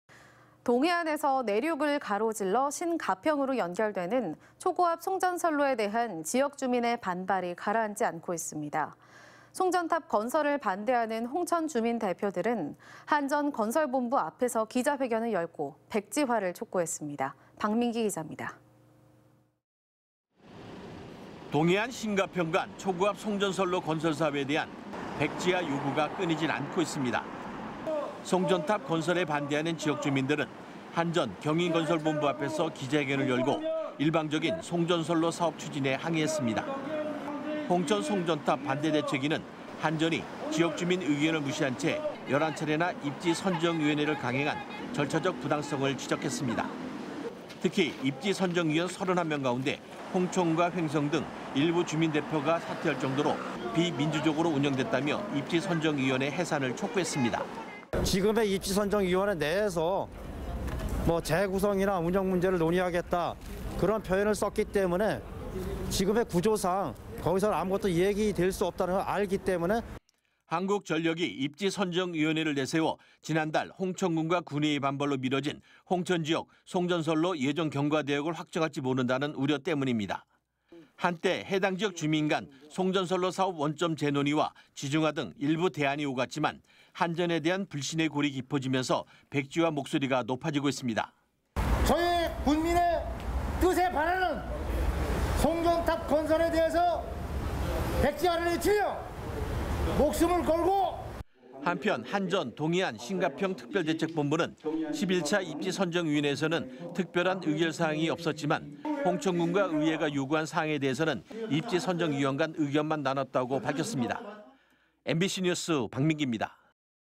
[춘천 MBC] 홍천 송전탑 반대특위..입지선정위 해산하라
◀ＡＮＣ▶
 동해안에서 내륙을 가로질러 신가평으로
연결되는 초고압 송전선로에 대한 지역주민의 반발이 가라앉지 않고 있습니다.

 송전탑 건설을 반대하는 홍천 주민대표들은
한전 건설본부 앞에서 기자회견을 열고
백지화를 촉구했습니다. 박민기 기잡니다.

           ◀ＥＮＤ▶
           ◀ＶＣＲ▶

 동해안-신가평 간 초고압 송전선로 건설사업에 대한 백지화 요구가 끊이질 않고 있습니다.

 송전탑 건설에 반대하는 지역주민들은
한전 경인건설본부 앞에서는 기자회견을 열고 일방적인 송전선로사업 추진에 항의했습니다.

 홍천송전탑 반대대책위는 한전이 지역주민
의견을 무시한채 11차례나 입지선정위원회를
강행한 절차적 부당성을 지적했습니다.

 특히, 입지선정위원 31명 가운데 홍천과
횡성 등의 주민 대표가 잇따라 사퇴할 정도로 비민주적으로 운영됐다며 입지선정위원회
해산을 촉구했습니다.

◀ＩＮＴ▶ 남궁 석/홍천송전탑반대위원회 공동대책위원장 "지금에 입지선정위원회 내에서 재구성이나 운영 문제를 논의하겠다. 그런 표현을 썼기때문에 지금에 구조상 거기선 아무 이야기도 될 수 없단걸 알기에.."

 한국전력이 입지선정위원회를 내세워 지난 달 홍천군과 군의회의 반발로 미뤄진 홍천지역
송전선로 예정경과대역을 확정할 지 모른다는 우려 때문입니다.

 한때 해당지역 주민 간 송전선로사업 원점
재논의와 지중화 등 일부 대안이 오갔지만,
한전에 대한 불신의 골이 깊어지면서 백지화
목소리가 높아지고 있습니다.

◀ＩＮＴ▶ 김진옥/홍천송전탑반대위원회 공동대책위원장 "저희 군민의 뜻에 반하는 송전탑 건설에 대해서 백지화를 외치며 목숨을 걸고.."

 한편, 한전 동해안-신가평특별대책본부는 11차 입지선정위원회에선 특별한 의결사항이
없었지만, 홍천군과 의회가 요구한 사항에
대해서는 입지선정위원간에 의견을 나눴다고만 밝혔습니다.

 MBC 뉴스 박민깁니다.
           ◀ＥＮＤ▶